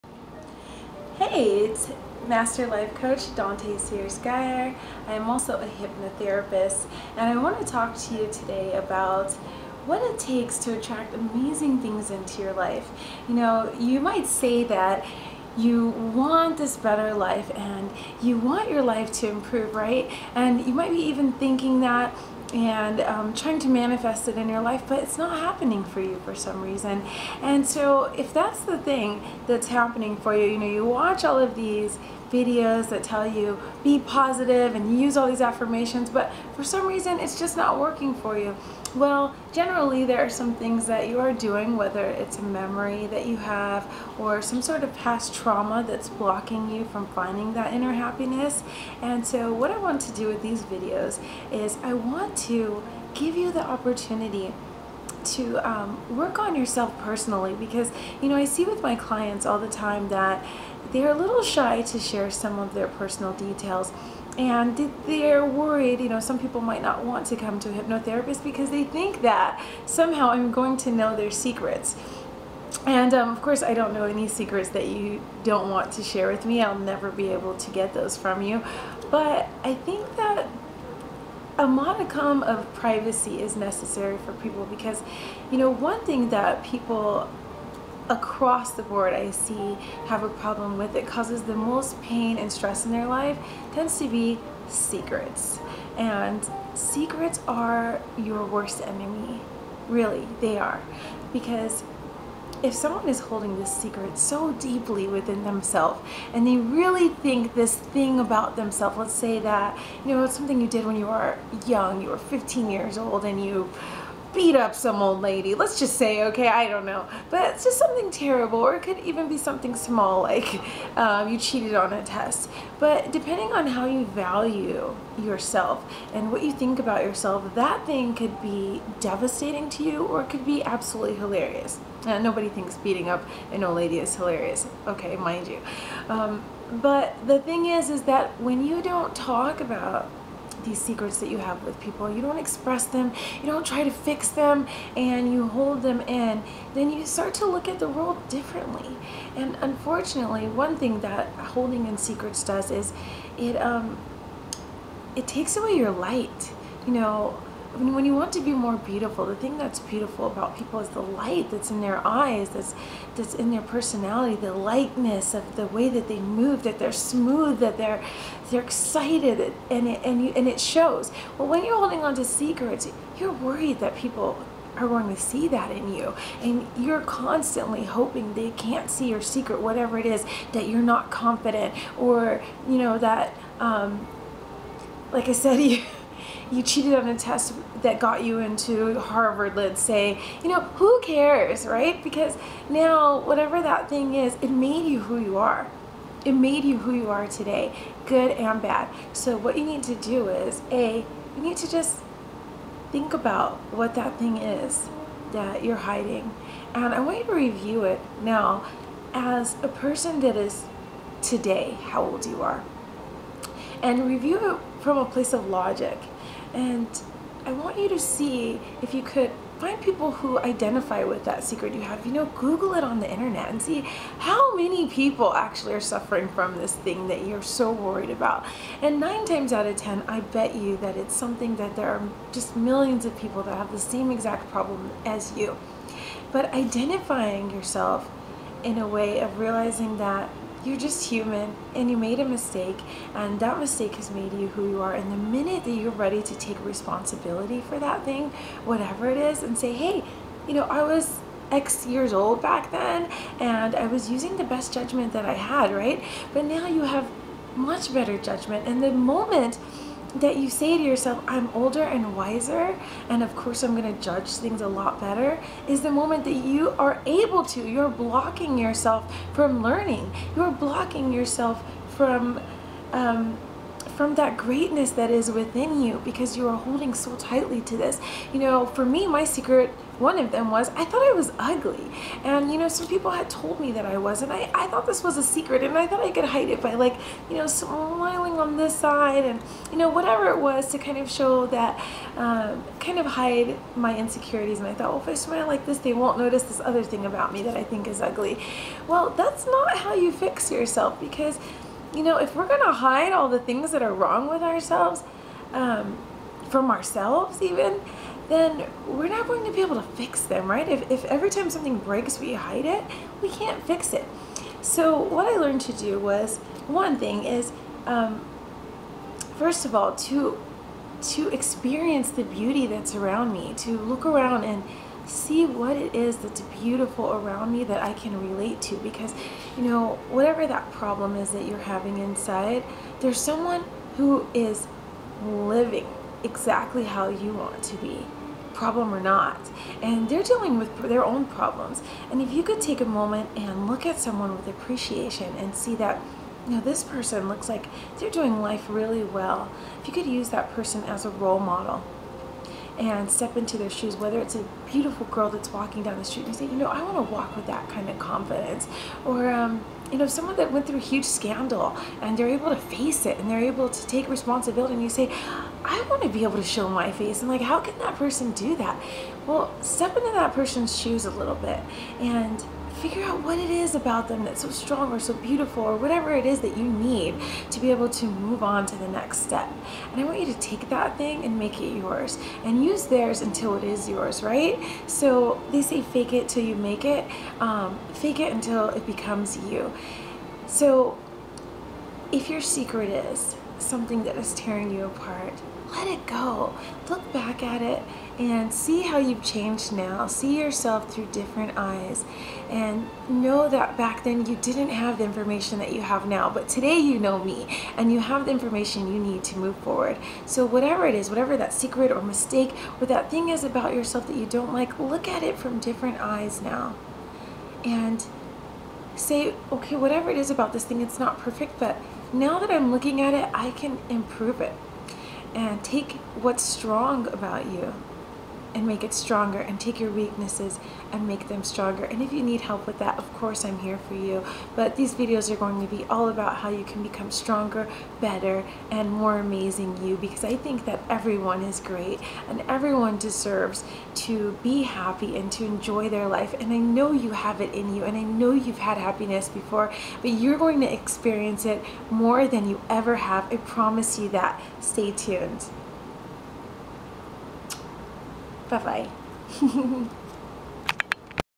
How to be Confident and Boost Self-Esteem - with Dante' Sears Geyer
What secret insecurities are holding you back from living a Happy, Successful, Love-filled life?  Certified Life Coach and Clinical Hypnotherapist, Dante Sears Geyer, discusses 3 key strategies that will reprogram your mind to Attract What You Want and to become more beautiful - inside and out.

Are you ready to Improve Your Life?